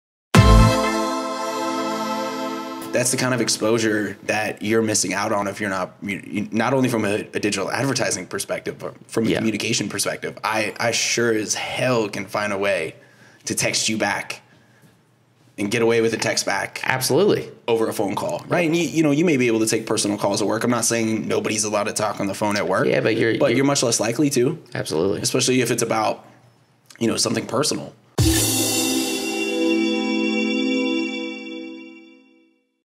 Texting Versus Calling
Are you missing out on leases because you're calling, rather than texting? 🤔

The REPLI360 team discusses on this week's episode of REPLITALK.